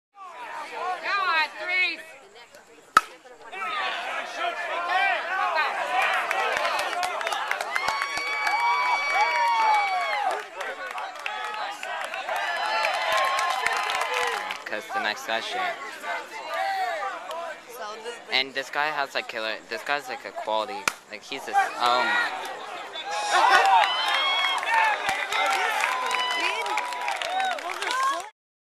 The Tony Kahan Collection - Volume I
Tony Kahan shows why he's a member of Team USA's Masters Softball, laying a beating on Canada at the 2009 Maccabi Games, Israel.